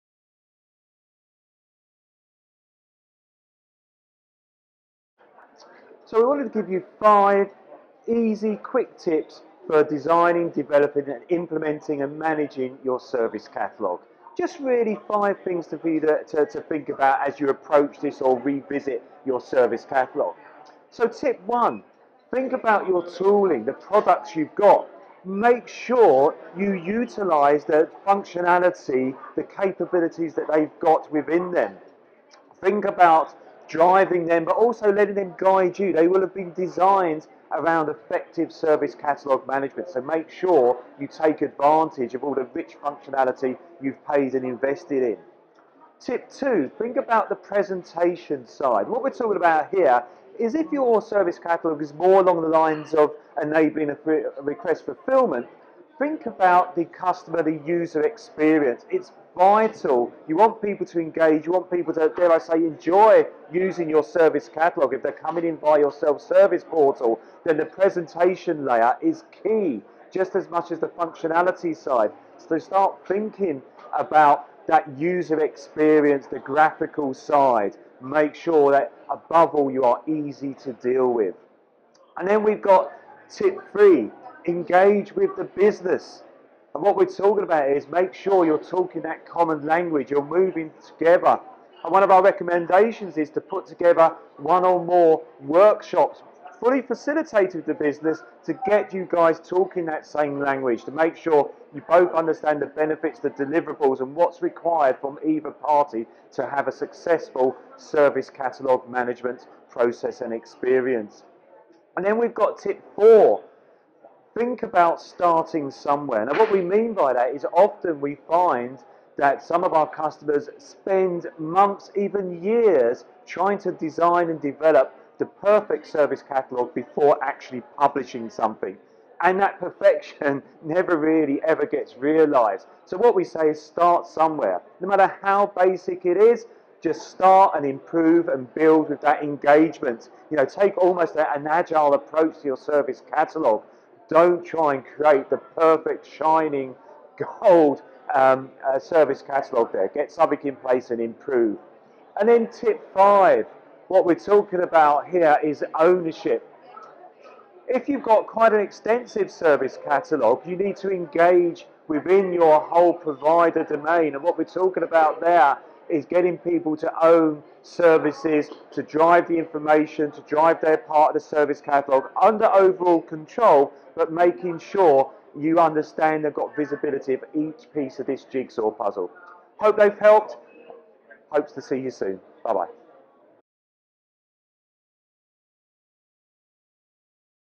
Service Catalogue Top Tip
We asked John Noctor to provide his Service Catalogue 'top tip'. If you want to learn more then John will be delivering two 1 day Service Catalogue Masterclass events during 2019 - please follow this link for event dates and details.